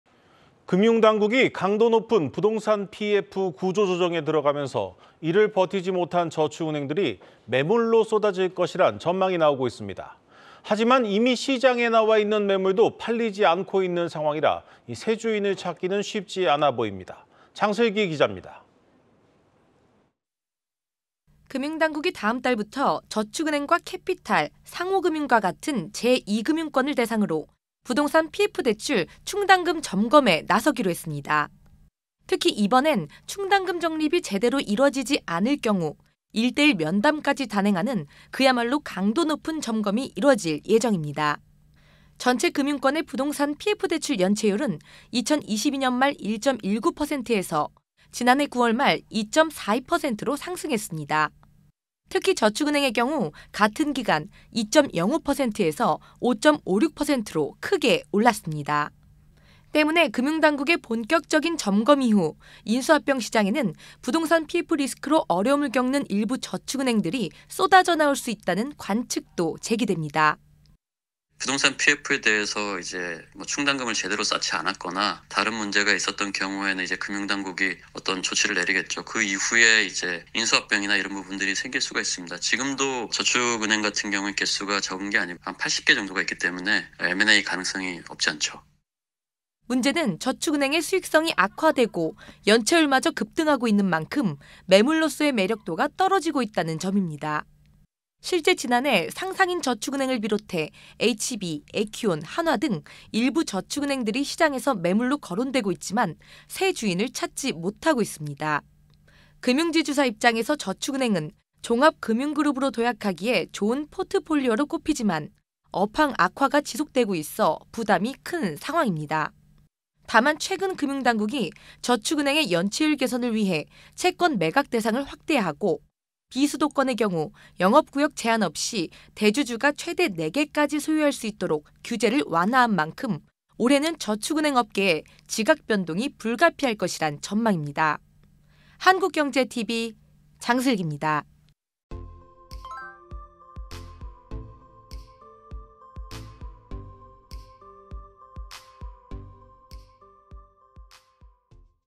저축은행 매물 쏟아져도…M&A는 '난항'/한국경제TV뉴스
금융당국이 강도 높은 부동산 PF 구조조정에 들어가면서, 이를 버티지 못한 저축은행들이, 매물로 쏟아질 것이란 전망이 나오고 있습니다. 하지만, 이미 시장에 나와있는 매물도, 팔리지 않고 있는 상황이라, 새 주인을 찾기는, 쉽지 않아 보입니다. 경제부 장슬기 기자입니다.